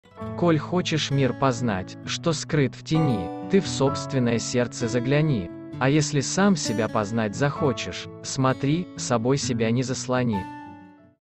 Мирза Шафи Вазех азербайджанский поэт 1794 - 1852, цитата.
Мирза Шафи Вазех — азербайджанский поэт и мыслитель, просветитель и педагог. Под псевдонимом Вазех, что означает «выразительный, ясный», писал как на азербайджанском, так и на персидском, развивая традиции поэзии на этих языках. Преподавал в Тифлисе азербайджанский и персидский языки.